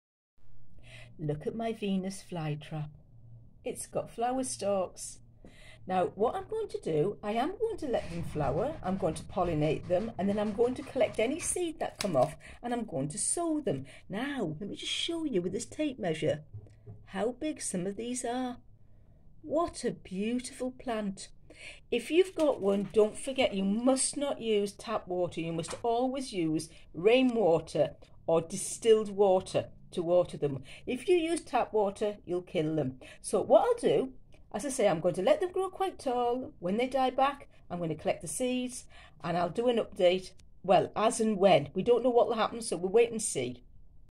Venus flytrap flower stalk
I've never propagated one before so this is purely an experiment.  Don't expect an update any time soon as I've no idea how long it will take.